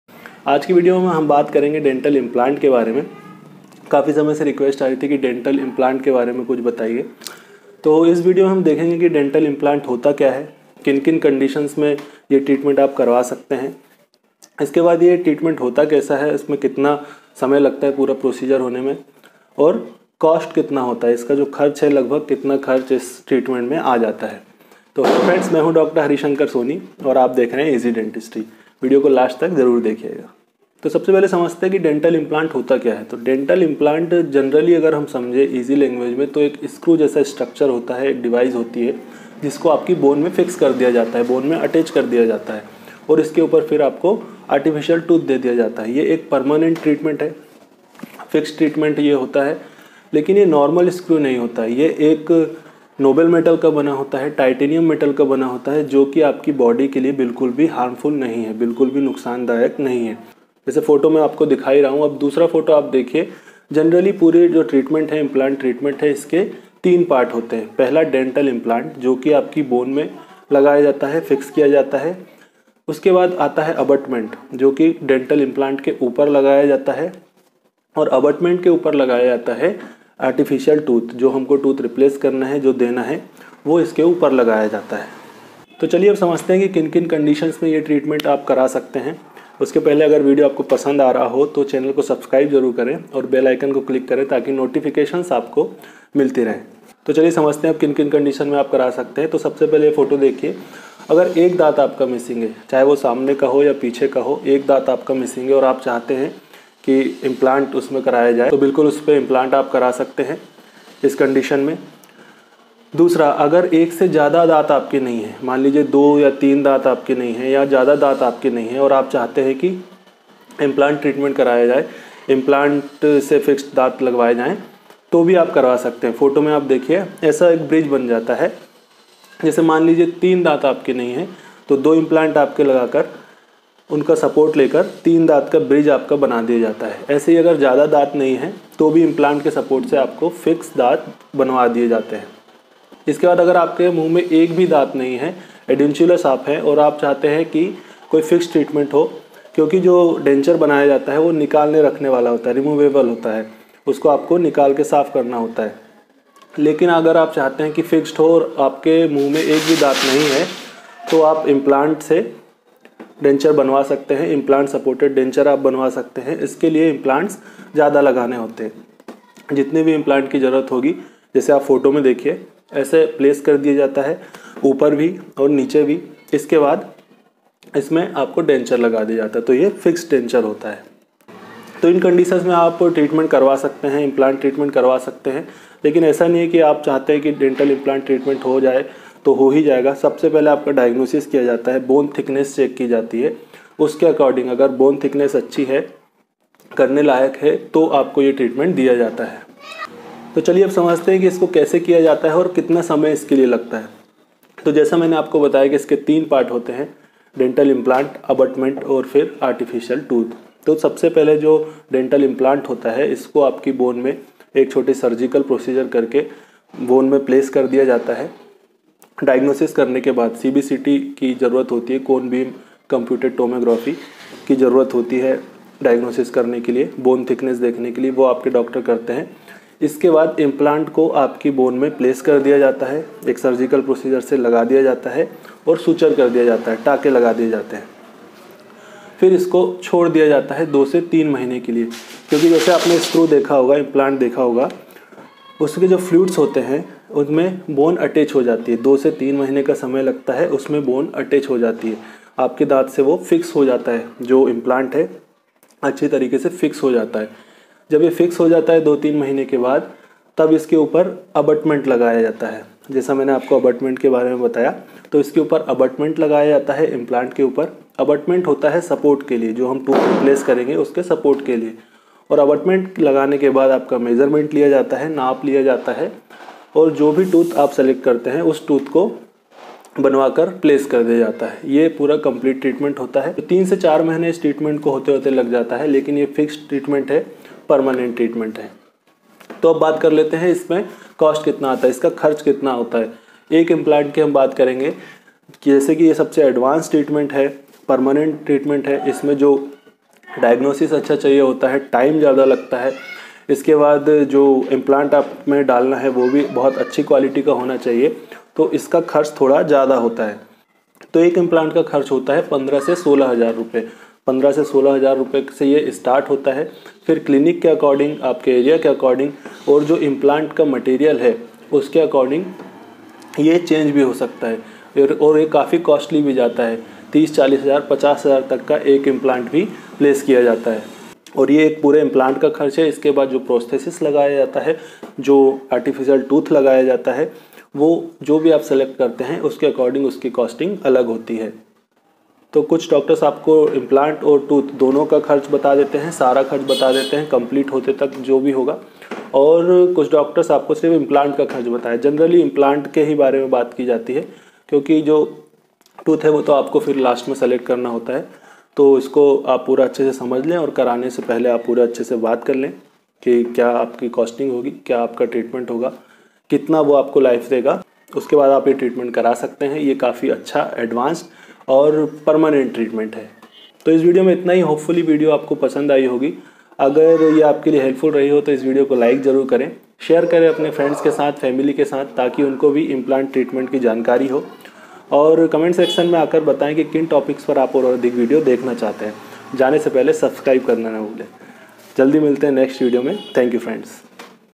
Dental Implant in Hindi | Cost , Procedure & Duration
This video is about dental implant..
This is advanced and permanent treatment for replacing missing tooth..

If you want to understand about dental implant, it's procedure and what is the approx cost of this treatment..must watch the video till end..